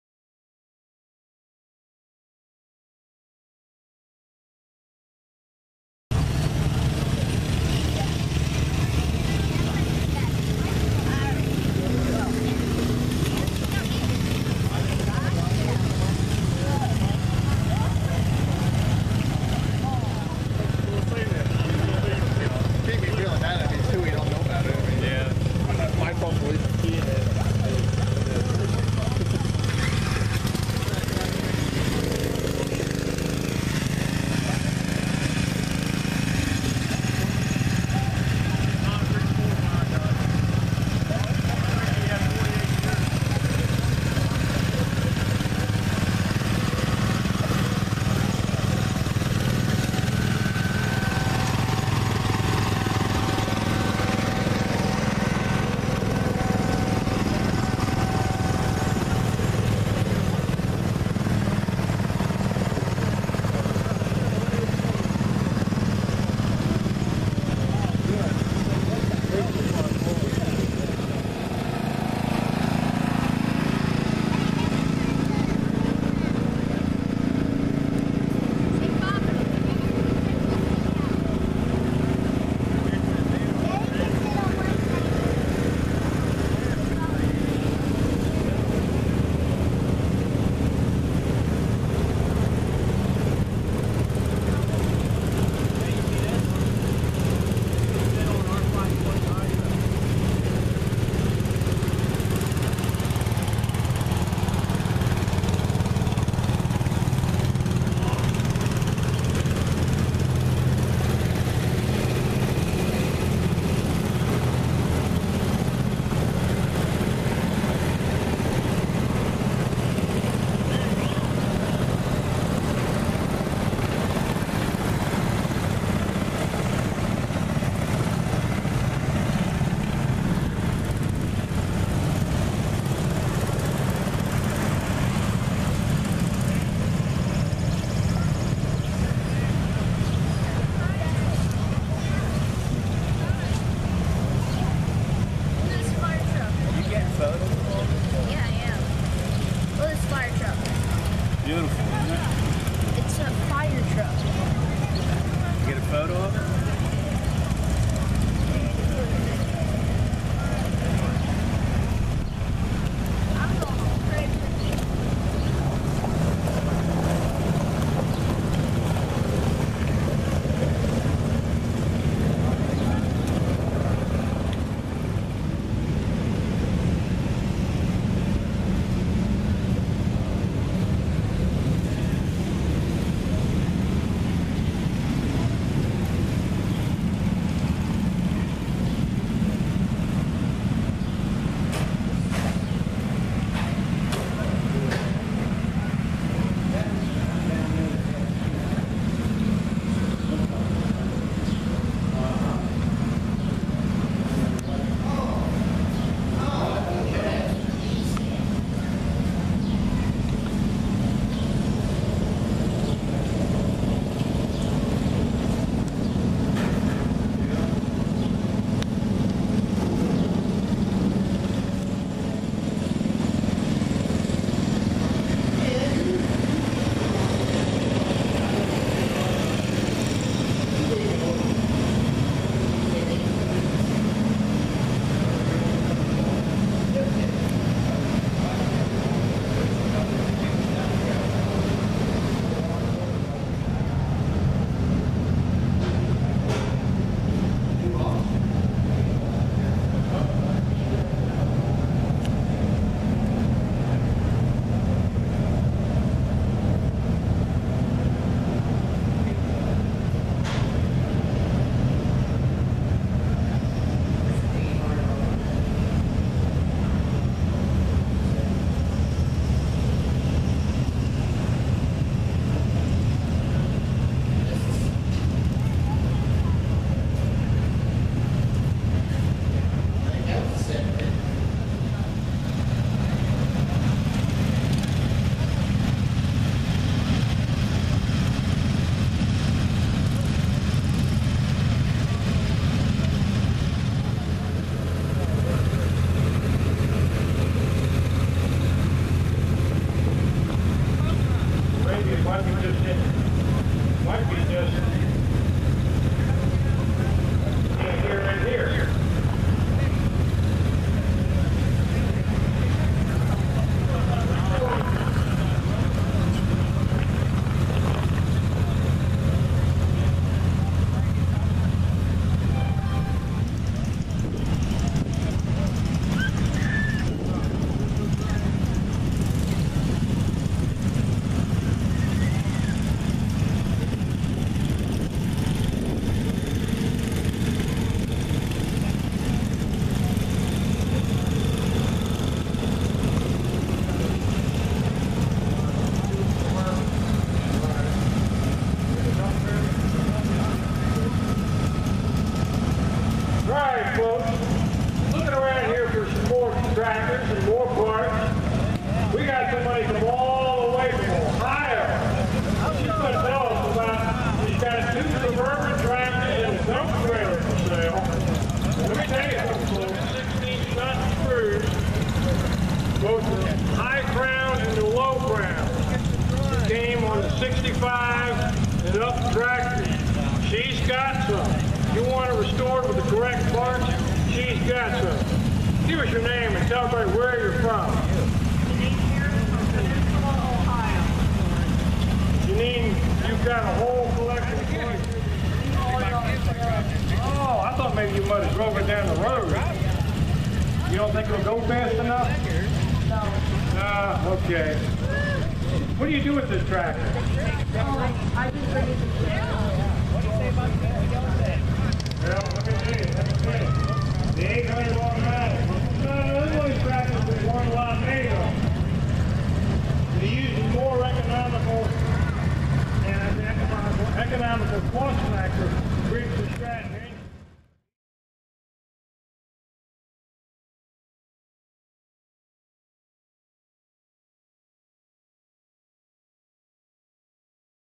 2017 PA Wheel Horse Show
Here are a few clips from the 2017 Wheel Horse show in Biglerville, PA. The featured tractor of this show was the 520H. Tons of Wheel Horse tractors on display, lots and lots of different attachments, and plenty of parts for sale. Check it out and be sure to comment if you see your tractor in this video!